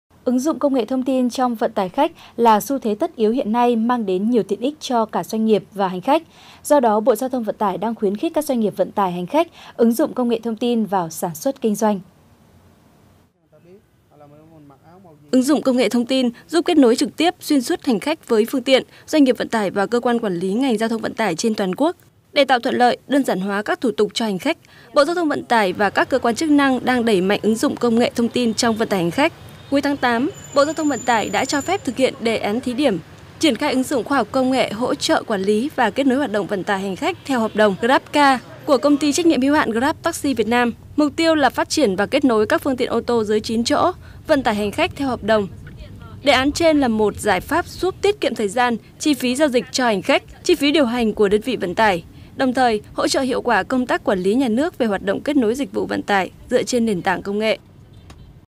VTC14_Bộ GTVT khuyến khích ứng dụng CNTT vào vận tải hành khách
(Truyền hình VTC14) - Ứng dụng công nghệ thông tin (CNTT) trong vận tải khách là xu thế tất yếu hiện nay mang đến nhiều tiện ích cho cả doanh nghiệp và hành khách. Do đó, Bộ GTVT đang khuyến khích các doanh nghiệp vận tải hành khách ứng dụng CNTT vào sản xuất, kinh doanh.